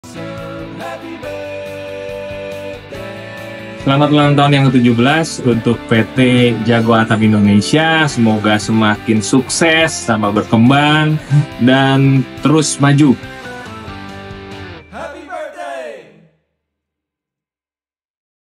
Ucapan Terima Kasih dari SI JAGO ATAP GROUP kepada PT. M CLASS INDUSTRY
SI JAGO ATAP GROUP ingin menyampaikan Terima kasih yang mendalam untuk PT. Keramika Indonesia Assosiasi (KIA) atas dedikasi dan komitmen yang telah ditunjukkan selama ini.

Semoga tahun-tahun mendatang membawa lebih banyak kesuksesan dan inovasi yang membanggakan. Terima kasih telah menjadi mitra yang berharga dan berkontribusi pada kemajuan industri ini.